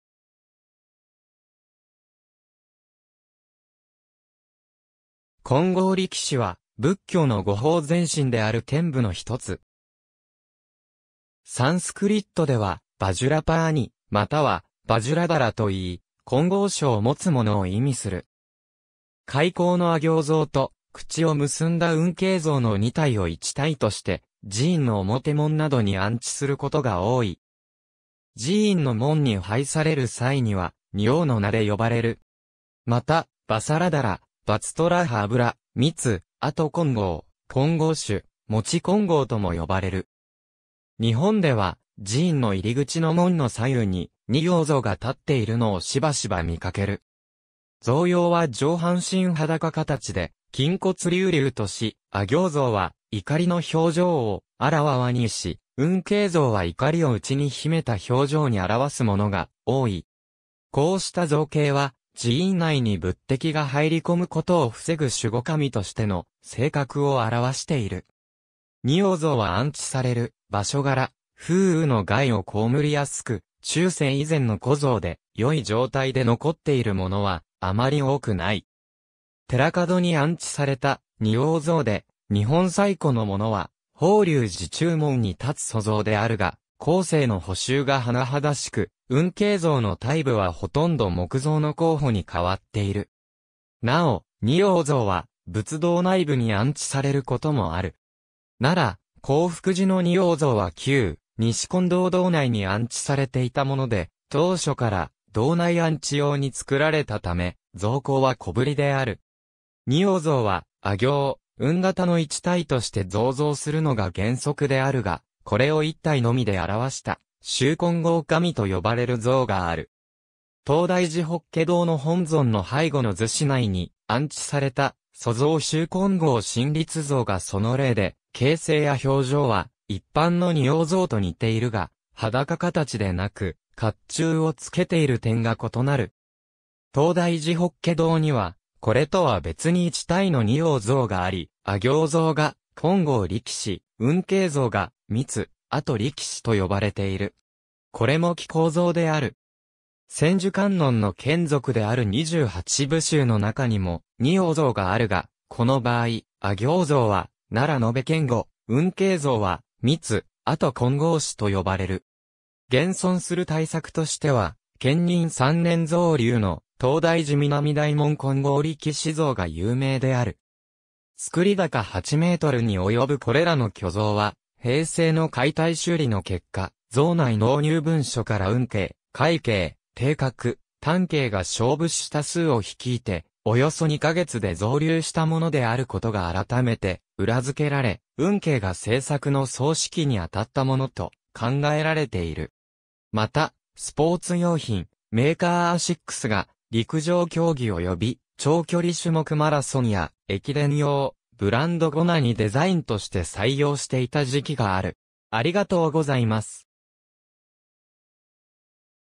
金剛力士
金剛力士（こんごうりきし、梵: वज्रपाणि、Vajrapāṇi）は、仏教の護法善神（守護神）である天部の一つ。
サンスクリットでは「ヴァジュラパーニ」、または「ヴァジュラダラ(Vajradhara)」といい「金剛杵（こんごうしょ、仏敵を退散させる武器）を持つもの」を意味する。
開口の阿形（あぎょう）像と、口を結んだ吽形（うんぎょう）像の2体を一対として、寺院の表門などに安置することが多い。
寺院の門に配される際には仁王（におう、二王）の名で呼ばれる。
また、伐折羅陀羅（ばざらだら）、跋闍羅波膩（ばじゃらぱに）、密迹金剛、金剛手（こんごうしゅ）、持金剛（じこんごう）とも呼ばれる。
日本では寺院の入口の門の左右に仁王像が立っているのをしばしば見かける。
像容は上半身裸形で、筋骨隆々とし、阿形像は怒りの表情を顕わにし、吽形像は怒りを内に秘めた表情に表すものが多い。
こうした造形は、寺院内に仏敵が入り込むことを防ぐ守護神としての性格を表している。
仁王像は安置される場所柄、風雨の害を蒙りやすく、中世以前の古像で、良い状態で残っているものはあまり多くない。
寺門に安置された仁王像で日本最古のものは法隆寺中門に立つ塑像であるが、後世の補修が甚だしく、吽形像の体部はほとんど木造の後補に変わっている。
なお、仁王像は仏堂内部に安置されることもある。
奈良・興福寺の仁王像（鎌倉時代、国宝）は旧・西金堂堂内に安置されていたもので、当初から堂内安置用に造られたため、像高は小ぶりである。
仁王像は阿形・吽形の一対として造像するのが原則であるが、これを1体のみで表した、執金剛神（しゅこんごうしん）と呼ばれる像がある。
東大寺法華堂（三月堂）の本尊の背後の厨子内に安置された塑造執金剛神立像（国宝）がその例で、形勢や表情は一般の仁王像と似ているが、裸形でなく甲冑を着けている点が異なる。
東大寺法華堂にはこれとは別に一対の仁王像があり、阿形像が「金剛力士」、吽形像が「密迹力士」（みっしゃくりきし）と呼ばれている。
これも着甲像である。
千手観音の眷属である二十八部衆の中にも仁王像があるが、この場合、阿形像は「那羅延堅固」（ならえんけんご）、吽形像は「密迹金剛士」（みっしゃくこんごうし）と呼ばれる。
現存する大作としては建仁3年（1203）造立の東大寺南大門金剛力士（仁王）像が有名である。
造高8メートルに及ぶこれらの巨像は、平成の解体修理の結果、像内納入文書から運慶、快慶、定覚、湛慶（運慶の子）が小仏師多数を率いておよそ2か月で造立したものであることがあらためて裏付けられ、運慶が制作の総指揮にあたったものと考えられている。
また、スポーツ用品メーカー アシックスが、陸上競技 及び長距離種目 マラソンや駅伝用ブランド GONA にデザインとして採用していた時期が在る。